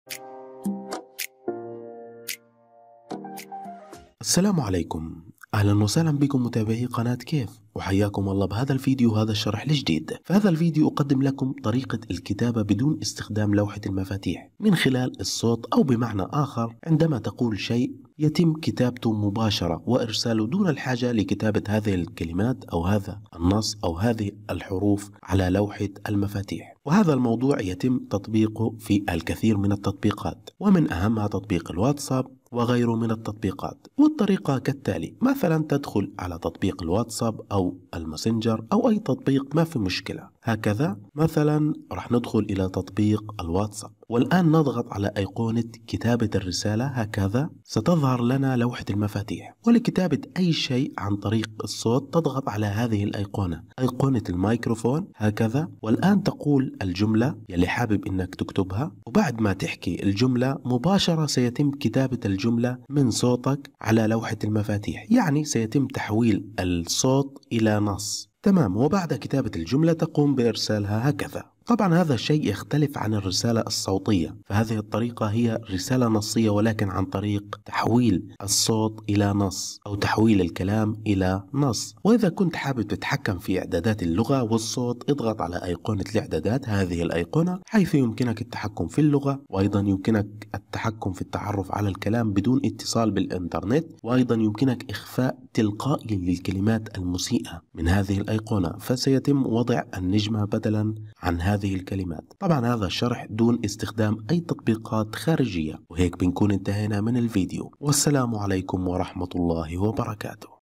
طريقة تحويل الصوت الى نص مكتوب في الهاتف بدون برامج
في هذا الفيديو نشرح لكم كيفية تحول الصوت الى نص في الهاتف بدون إستخدام برامج باستخدام عدة طرق
مشاهدة طيبة .